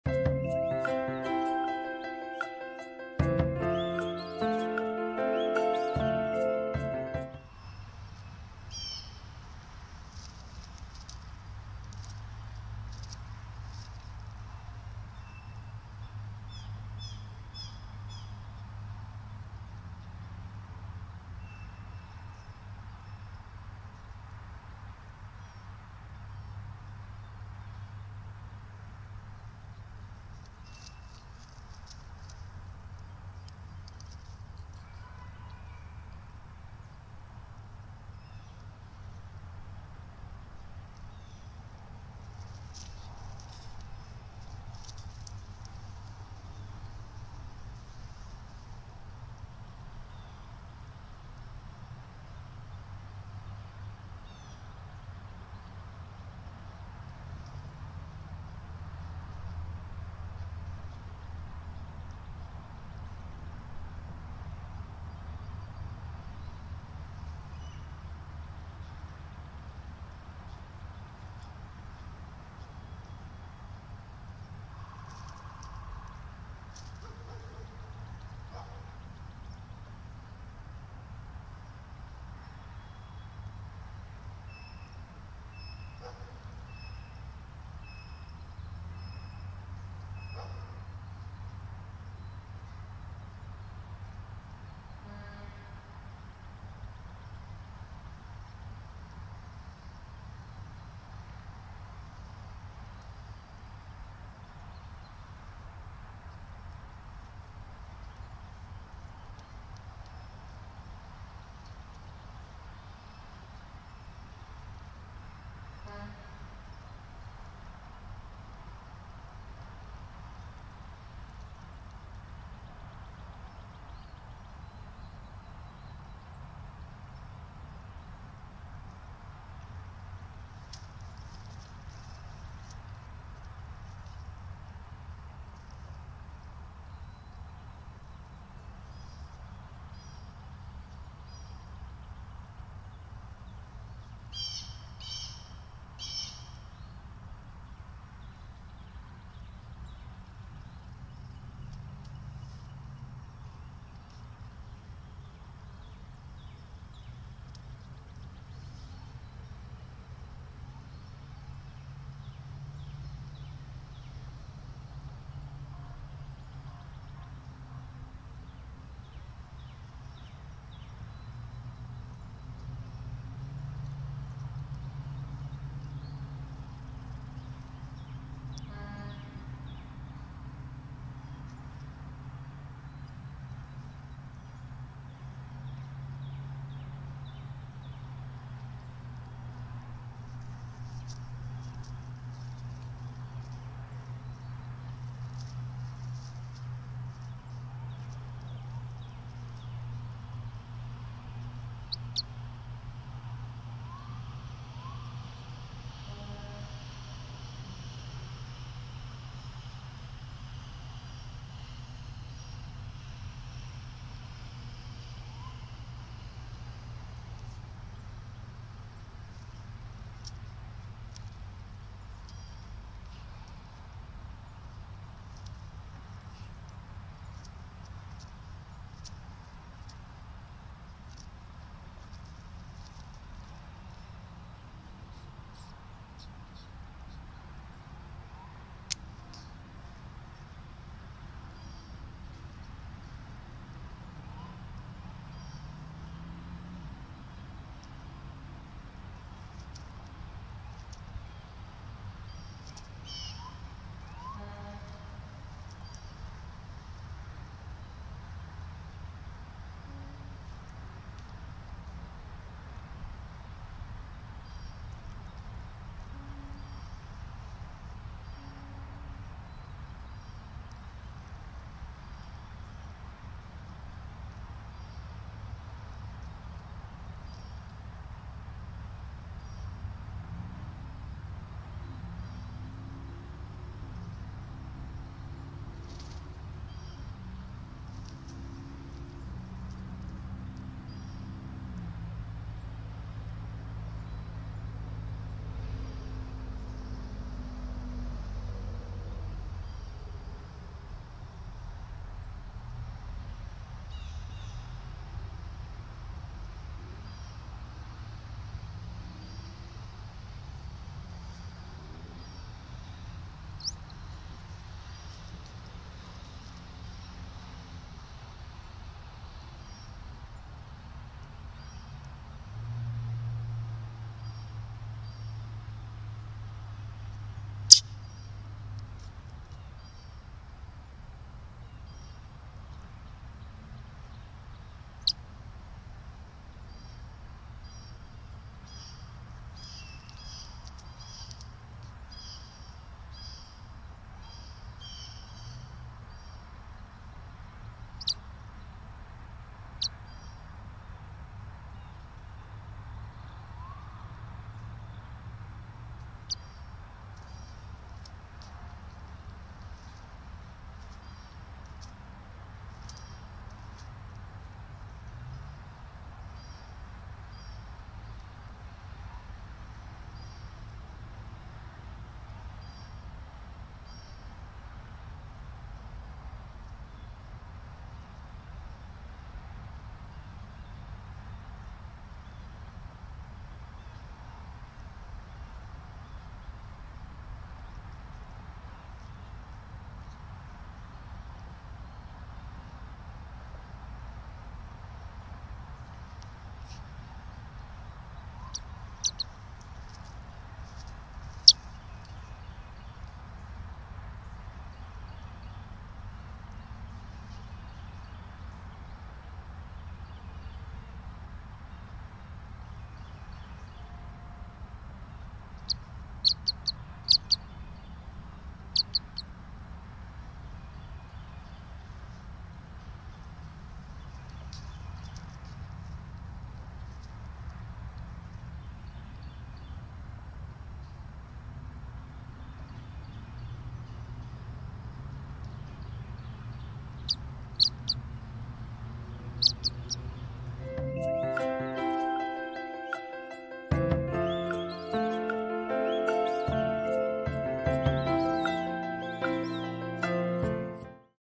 Home Alone & Being Cute - ETSU Johnson City Nest of Boone, Jolene, JC23 & JC24 (3/16)
JC23 & JC24 are enjoying more independence and today they spent some "home alone" time just being chummy with each other and being cute. Growing so quickly, these two cuties will both turn three weeks old over the next few days!

JC23 Hatched 2/25
JC24 Hatched 2/27

Screen Rec: Johnson City Cam ETSU Eagle Camera (3/16/24)

to browse my collection of handmade jewelry and crochet products.